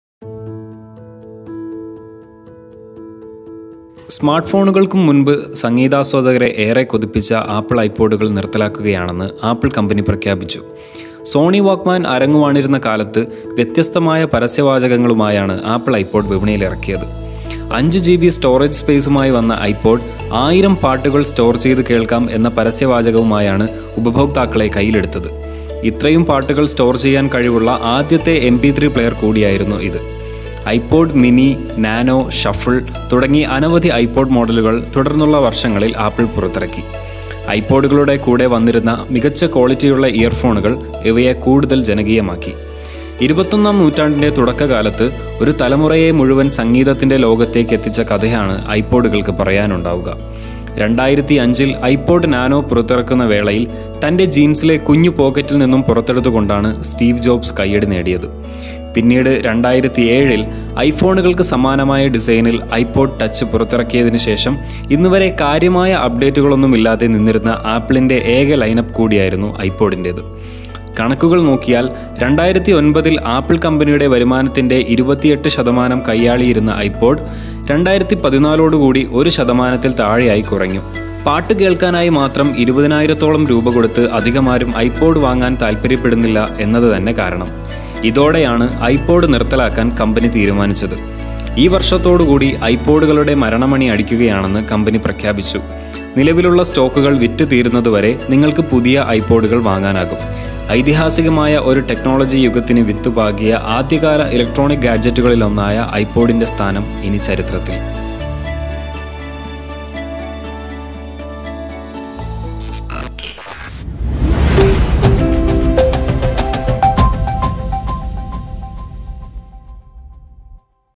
ഐപോഡ് യുഗം അവസാനിച്ചു | End of the iPod Era After Two Decades | NowNext
Apple finally discontinues the iPod Touch, the last available iPod models, since they are not generating enough monetary benefits for the trillion-dollar company.

NowNext | Career, Education and Entrepreneurship News in Kerala